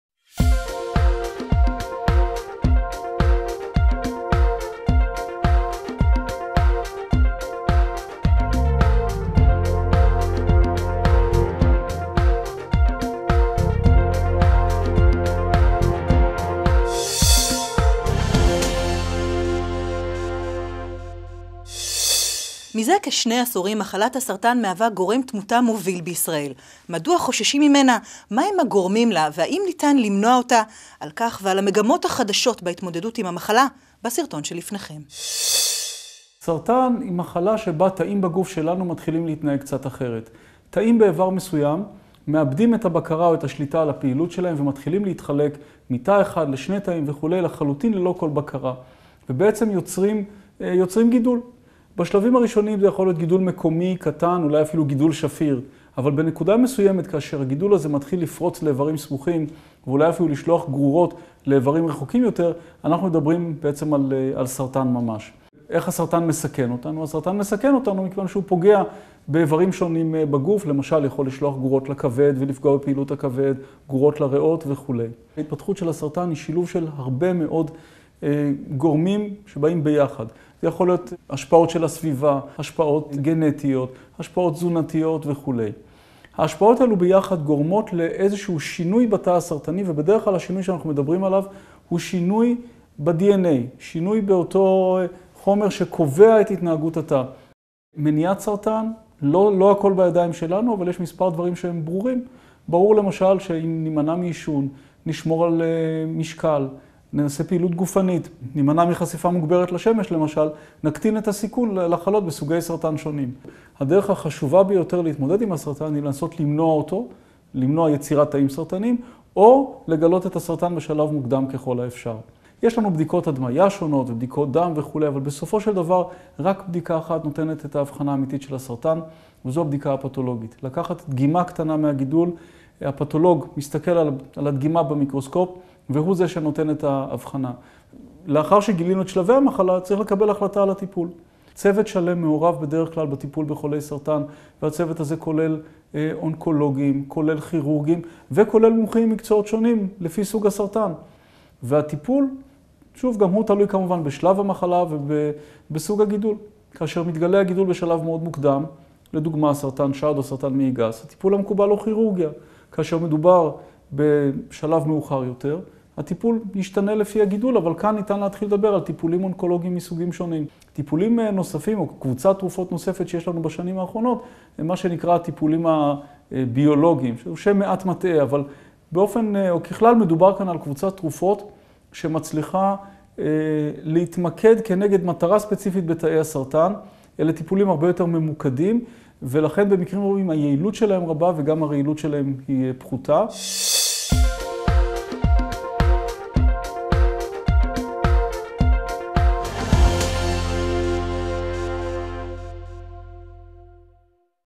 סרטן: שם כולל למגוון מחלות
יש הרואים במחלת הסרטן את האויב מספר אחת של האנושות בשל העובדה שהיא גובה קורבנות לא מעטים. רזון, איבוד שיער, אשפוזים והיעדרות ממושכת מעבודה בשל טיפולים,  מאפיינים את חולי הסרטן וחושפים אום בפני שאר העולם. בעיני רוב הציבור מחלת הסרטן מעוררת חששות. כולנו מכירים את המינוח "סרטן", אך מעטים מאיתנו יודעים באמת מהי אותה מחלה או קבוצת מחלות תחת כותרת זו. כיצד נוצרת מחלת הסרטן? כיצד היא מאובחנת? כיצד היא מתבטאת? האם ניתן למנוע אותה או לטפל בה?
ד"ר עידו וולף, מומחה לאונקולוגיה, מנהל אונקולוגיה רפואית, המערך האונקולוגי, במרכז הרפואי סוראסקי תל-אביב (איכילוב) מסביר אודות המחלה ואודות מגמות חדשות של התמודדות עימה ואף מנפץ מספר מיתוסים הסובבים סביבה.